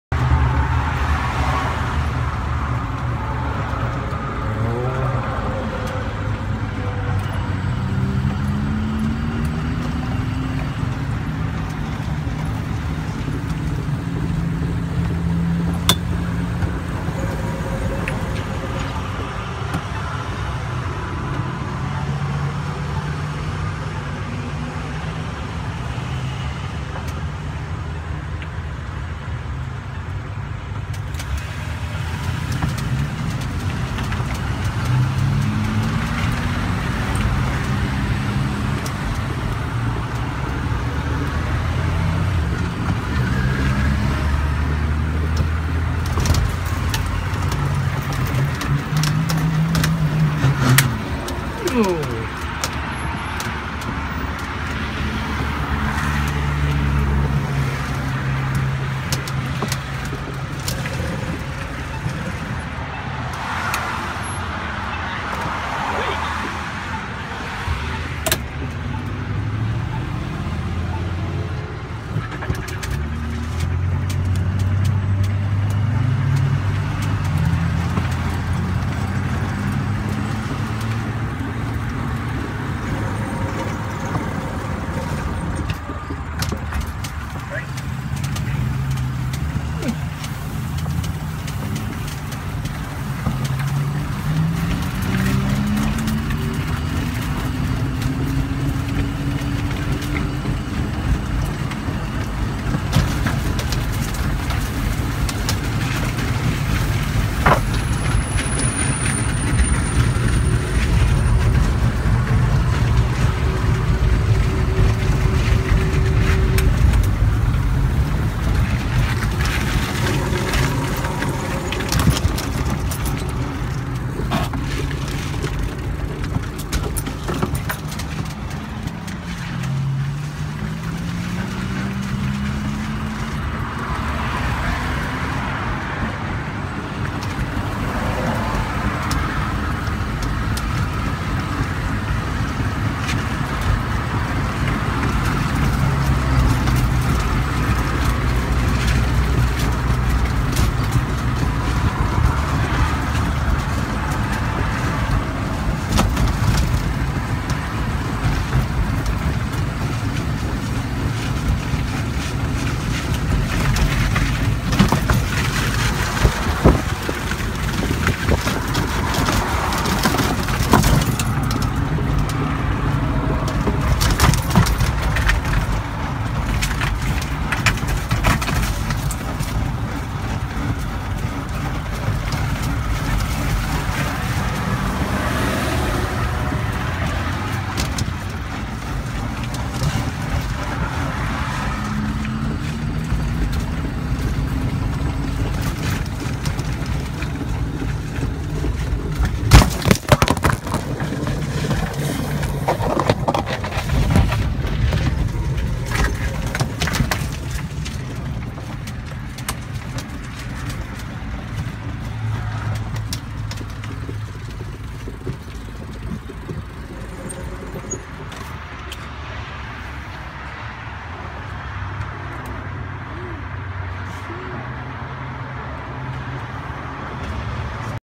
ELF Flipped Upside Down
An unedited live video of ELF on a run, hitting a bump unexpected and look what happened next. Watch and enjoy!

(Recorded 9/9/2016 @ 2:45pm)